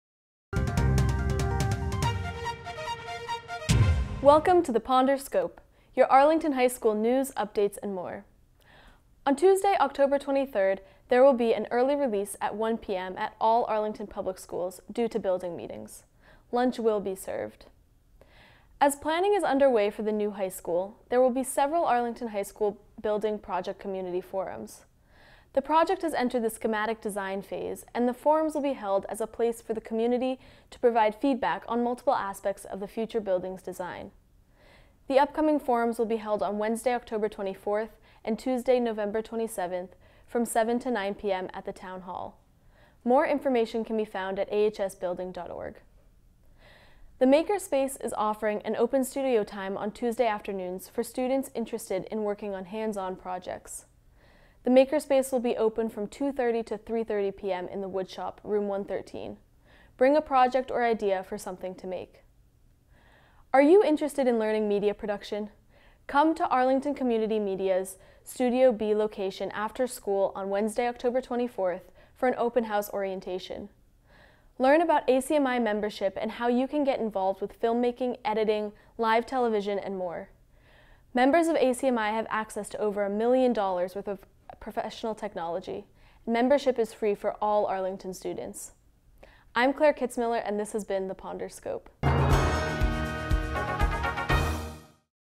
APN | The Ponder Scope | October 19, 2018
AHS Rebuild Forums, Maker space, and an ACMi open house on this weeks Ponder Scope.